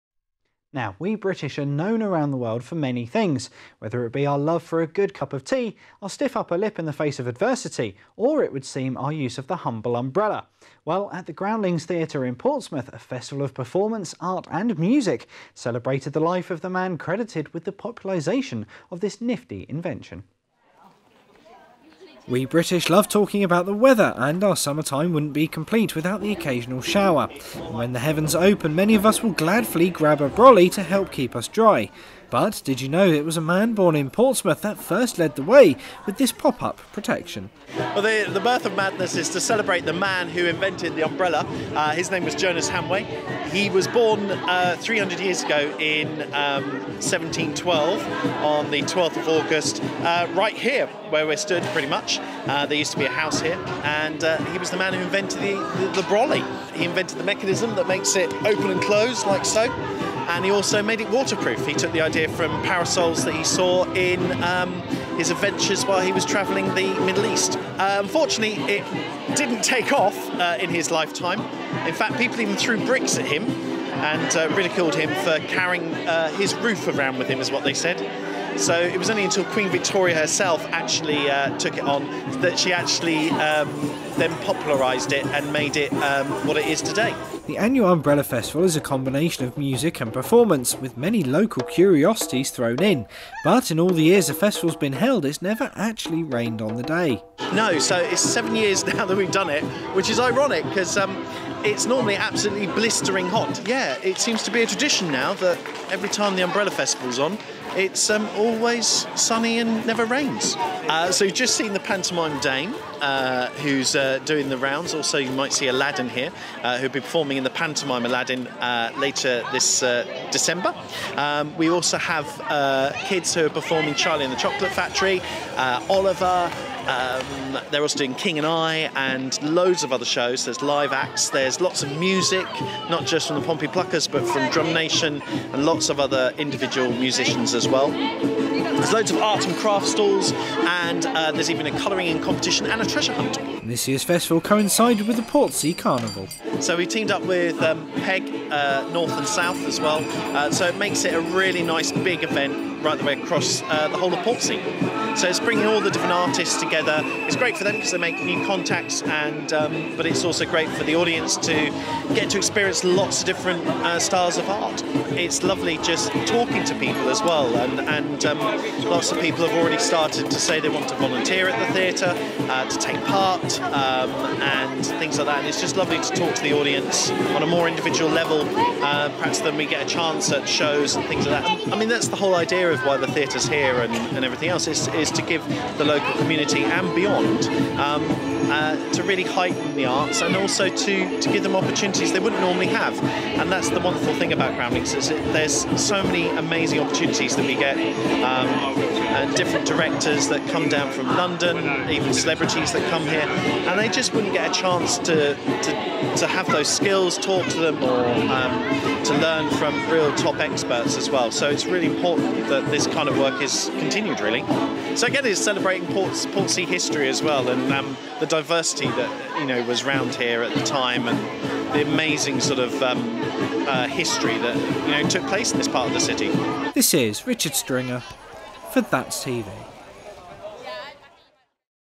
A Festival In Portsmouth Celebrates The Man Who Invented The Umbrella
We british are know around the world for many things, whether it be for our love of a good cup of tea, our stiff upper lip in the face of adversity, or it would seem our use of the humble umbrella.
 Well at the Groundlings theatre in Portsmouth, a festival of performance, art and music celebrated the life of the man credited with the popularisation of this nifty invention.